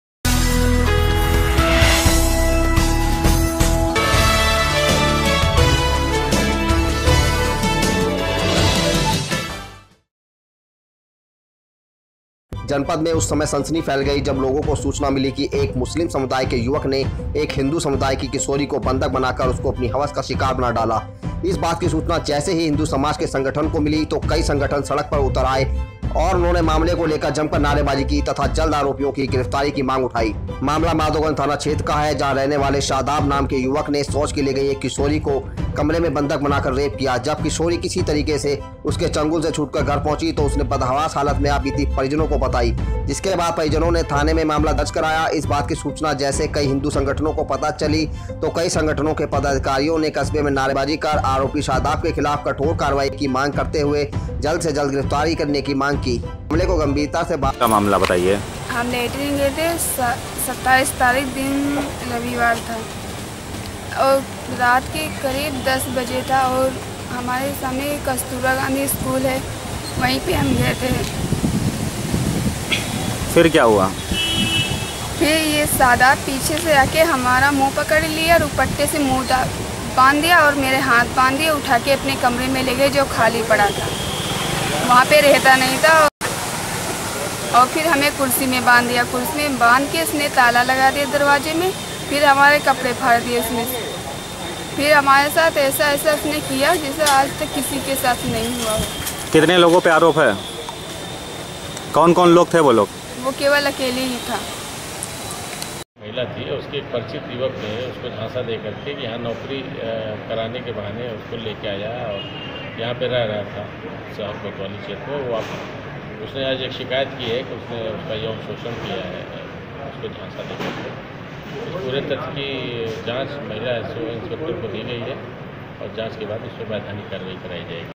मुस्लिम युवक ने हिन्दू लड़की को बनाया हवस का शिकार -Today24
जनपद में उस समय सनसनी फैल गई जब लोगो को सूचना मिली कि एक मुस्लिम समुदाय के युवक ने एक हिन्दू समुदाई की किशोरी को बंधक बनाकर उसको अपनी हवस का शिकार बना डाला है।इस बात की सूचना जैसे ही हिन्दू समाज के संगठनों को मिली तो कई संगठन सड़क पर उतर आए और उन्होंने मामले को जमकर नारेबाजी की तथा जल्द से आरोपी के ऊपर कठोर कार्यवाही की मांग की।

T.V. News

Application.

views!!!

News."